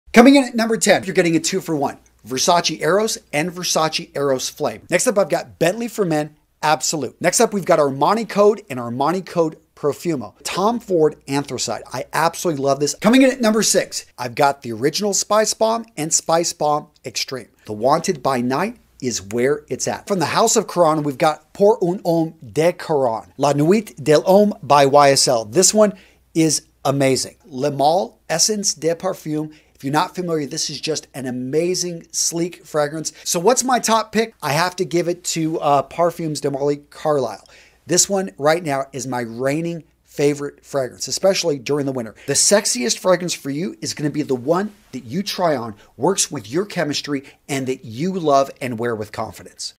Sexiest Colognes For Men (10 Must Try Fragrances!)
In today's video, I give you my 10 must try sexiest fragrances for men. Did I miss your favorite cologne from the list?